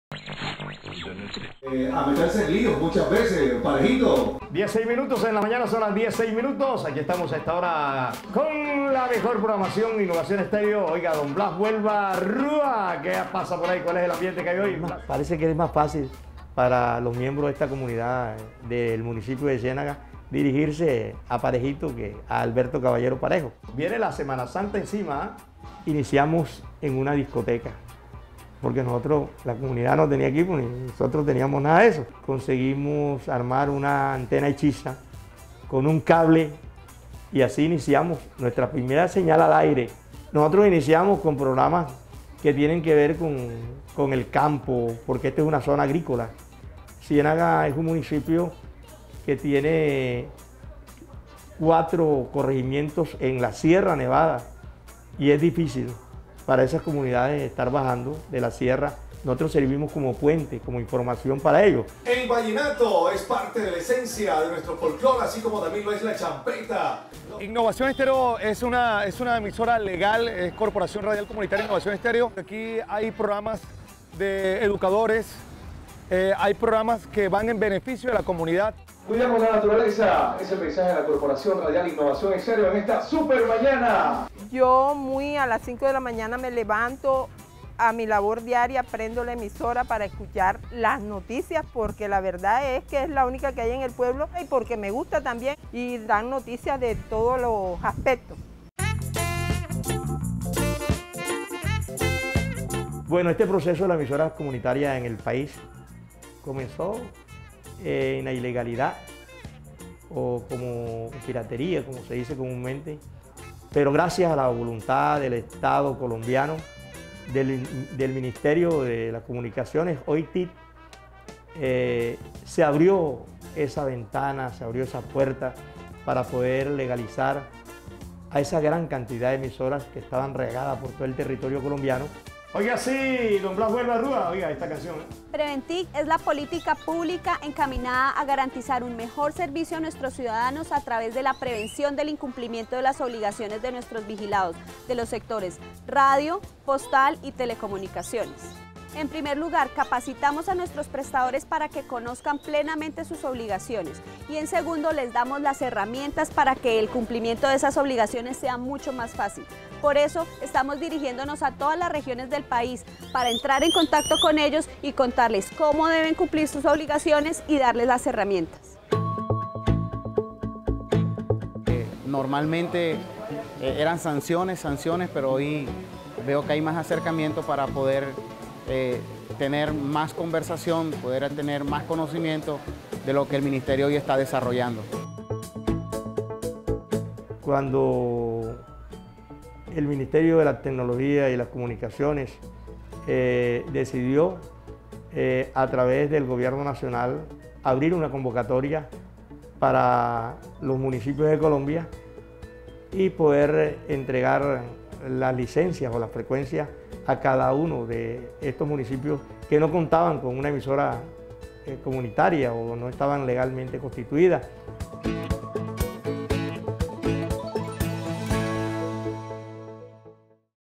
La Radio comunica y une a la población de Magdalena ViveDigitalTV C12-N6
Innovación Estéreo: emisora comunitaria informa, divierte y une a las personas de Magdalena.